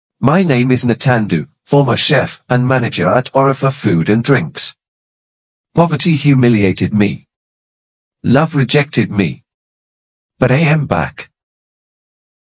THE RETURN OF NATANDU, THE MOST HANDSOME CHEF - WAIT FOR IT!
From going on a date with his boss Lady B to finding himself at shore of Canada for further studies. But now, Natta is back.
Wait for this mind bursting story. -  COMING SOON!

Ideka Stóries

african folktale, african folktales story, african folktales with moral lessons, african folktales.